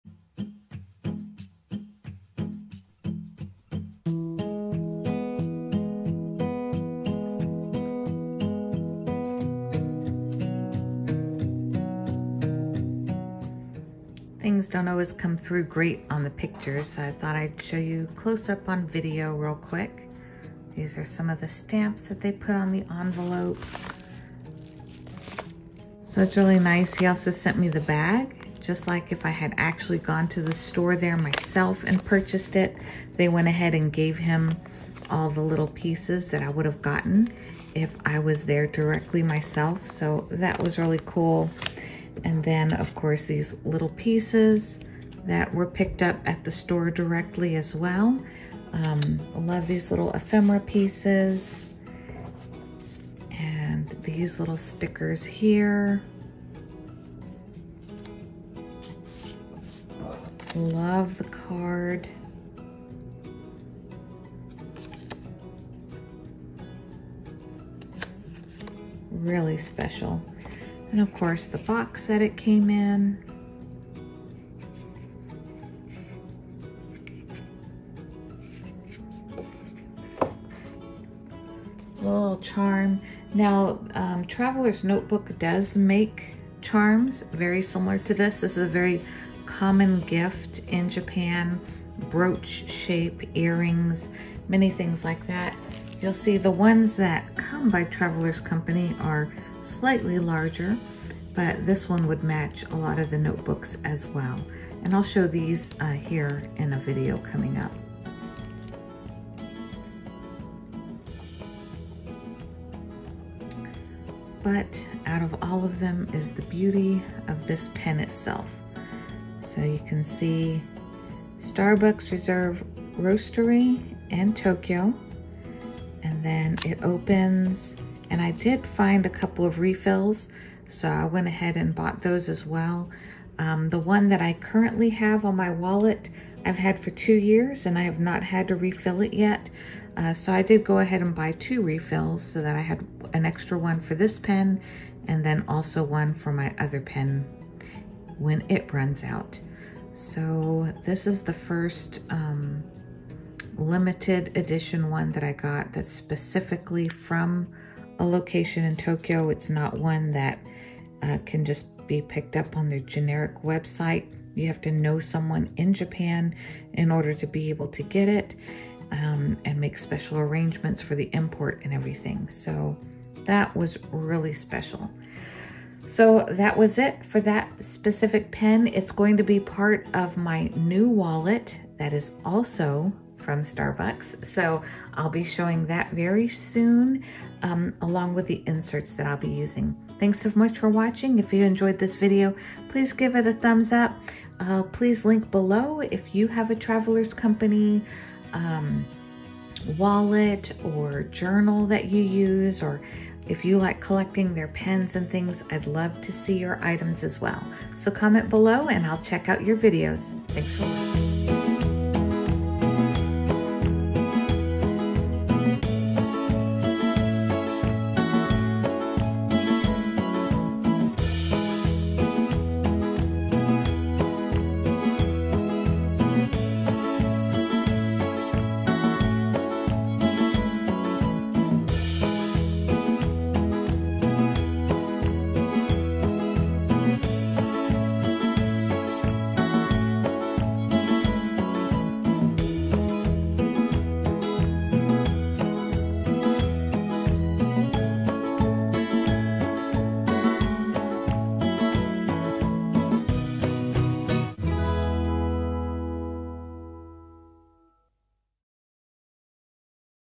Traveler’s Company Brass Pen Unboxing | Starbucks Reserve Roastery Tokyo Edition
I have had my eye on this series by Traveler’s company for a couple of years but I didn’t want to commit until I knew for sure that I would use it.  So, I broke myself in with a Blue Traveler’s Passport Size notebook as my EDC wallet back in 2019.  Prior to this, I had only been using the standard sized traveler’s notebooks. It has been the most practical and useful wallet set up for me.  So, now that I know I will use a passport size, I decided to take the plunge for the Limited Edition Starbucks Reserve Roastery Tokyo Edition TN.  This pen was the last item to complete my collection.  I can’t wait to get this wallet setup!  Now that I’ve had a couple of years to use it (during a pandemic, too) I know what is working and where I would like improvements, so I did purchase some 2021 mid-year inserts to make it even more functional.  This pen will be the perfect addition to this wallet.  I look forward to using this set every day.  This pen is light and packs very compact.  It is shorter than the passport notebook. It’s the perfect length when posted.